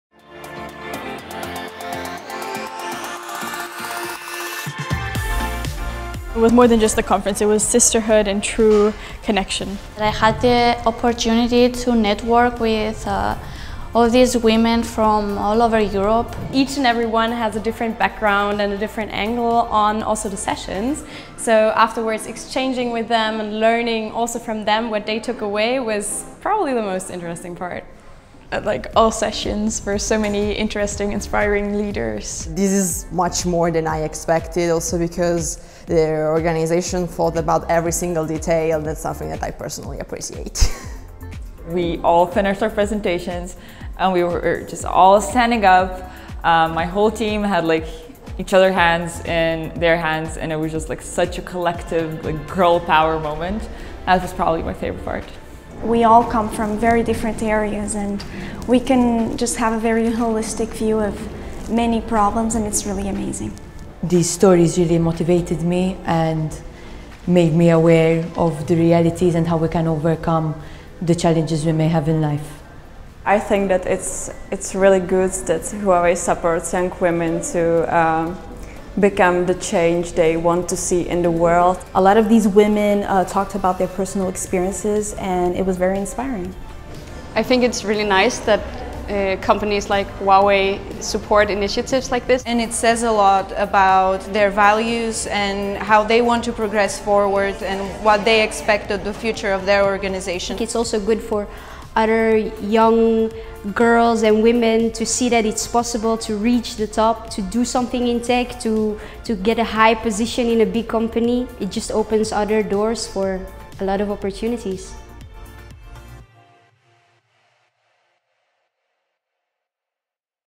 The Next Generation Change Makers / Summer School for Female Leadership in the Digital Age
Between 23 and 27 August 2021, Huawei held in Lisbon the first Summer School for Female Leadership in the Digital Age. From 1,225 applicants, an independent jury selected one participant from each of the 27 EU Member States to take part in this week-long programme of masterclasses, team projects, active learning and cultural experiences.

Some of the students shared their experience in this video.

There is infinite talent in Europe – but sometimes this talent is not well identified or supported and risks going to waste. To help women be more active participants in Europe’s recovery (a fundamental pillar of which is the digital transition), Huawei aims to recognise the immense potential of female talent, and to proactively support it, empowering girls to lead the tech revolution.